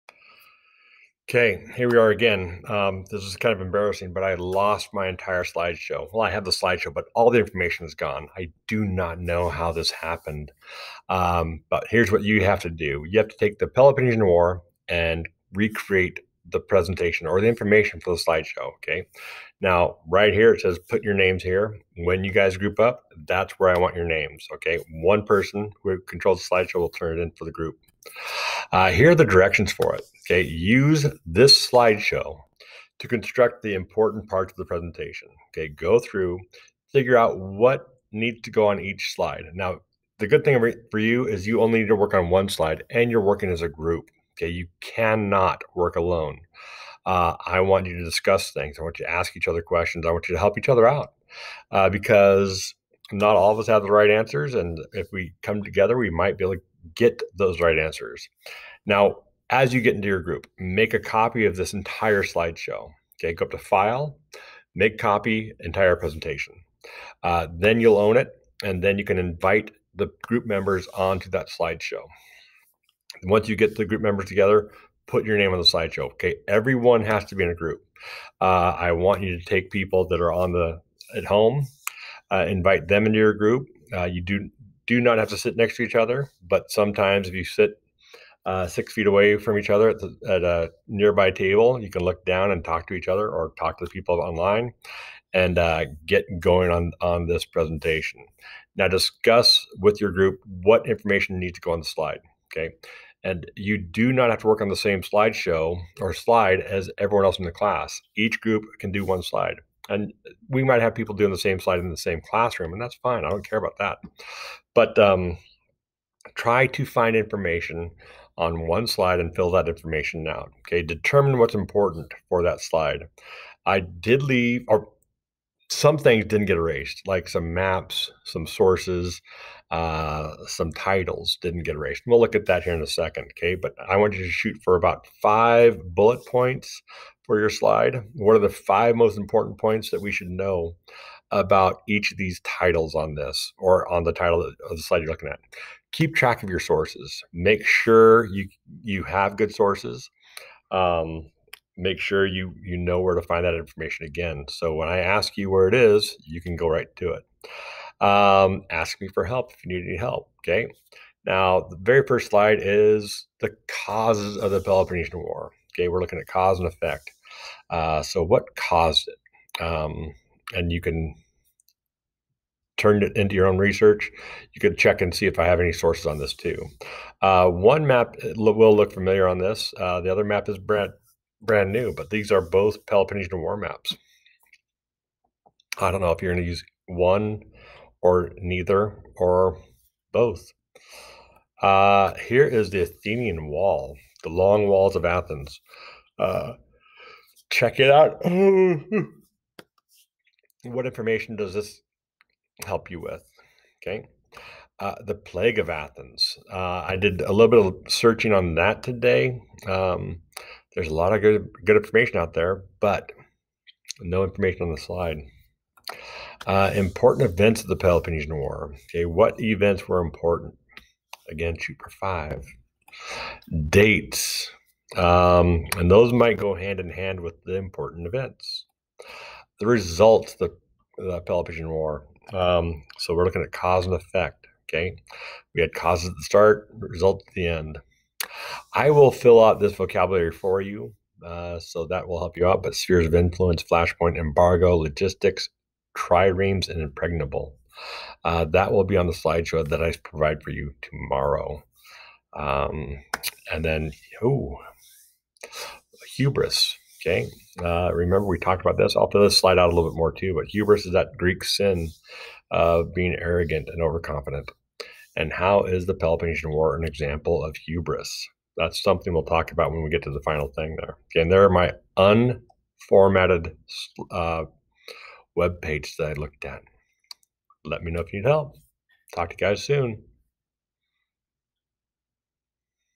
Peloponnesian War/The Lost Presentation
Sixth grade Social Studies: Group work activity to get the students to work in a group, discuss what important events are, decide sources to use and produce a quick project.  The presentation of this slide show will follow.